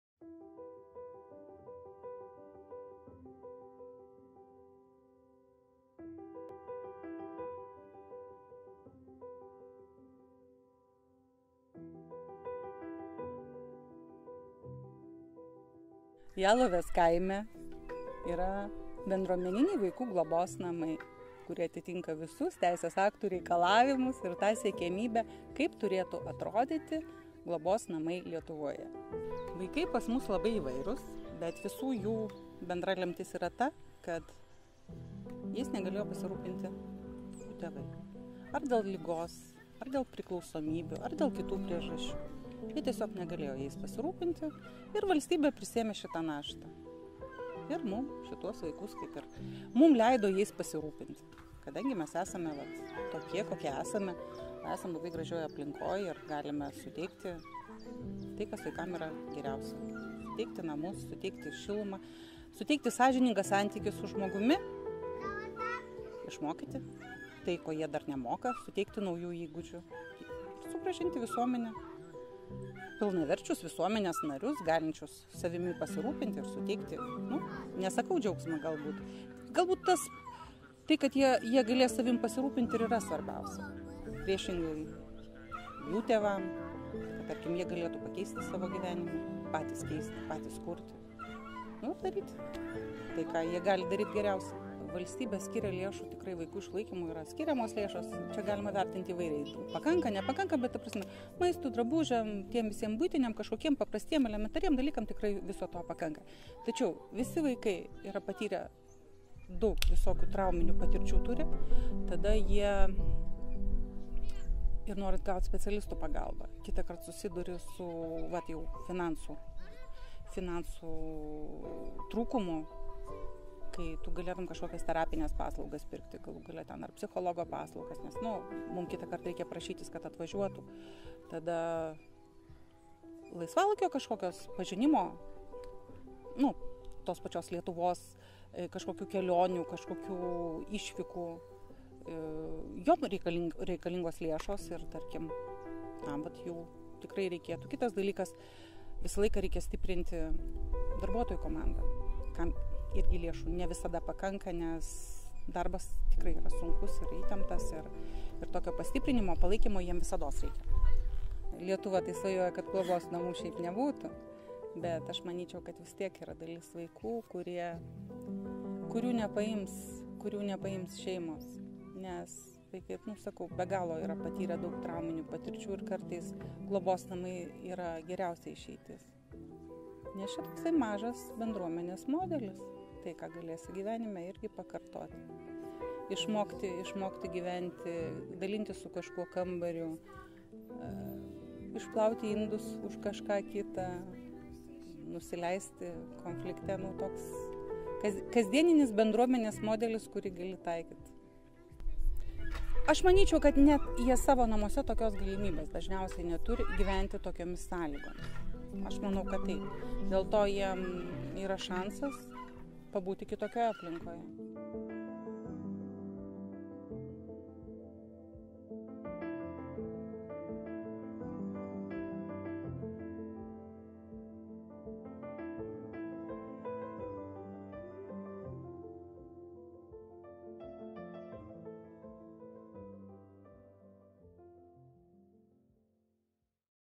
Švento Jono bendruomeniniai vaikų globos namai
Ligita Pabijūnienė pasakojo apie švento Jono bendruomeninius vaikų globos namus.